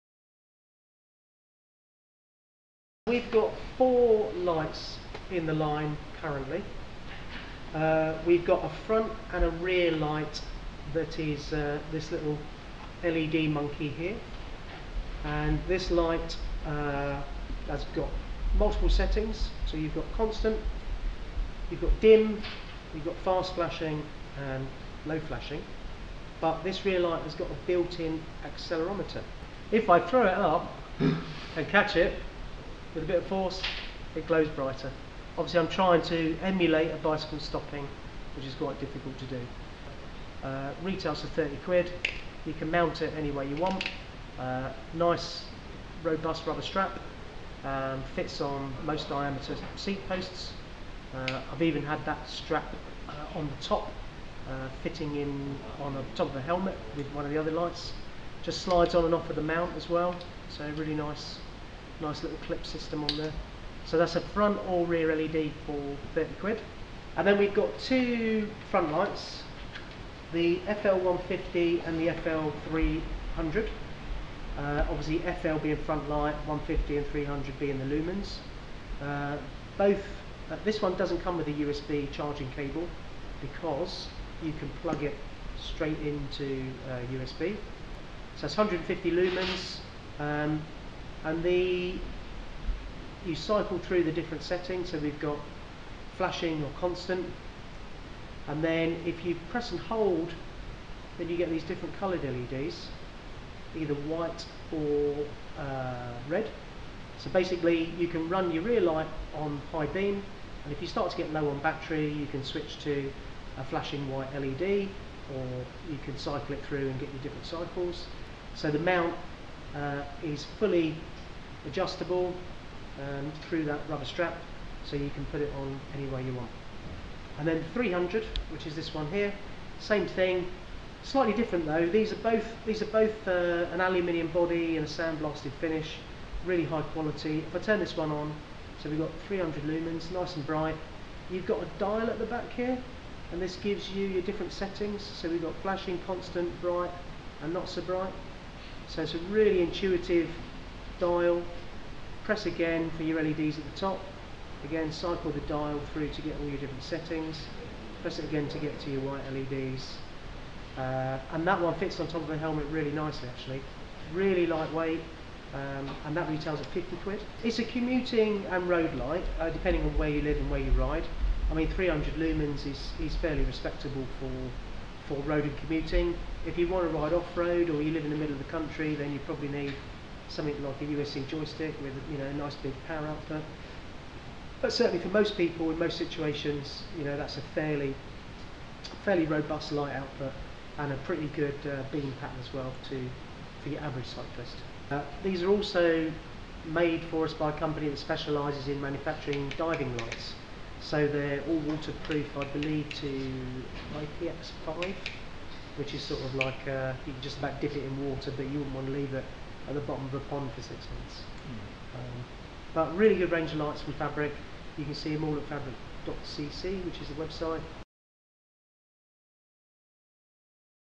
Fabric Lights
Clive Gosling talks to us about the new range of Fabric Lights, including a light featuring an accelerometer function.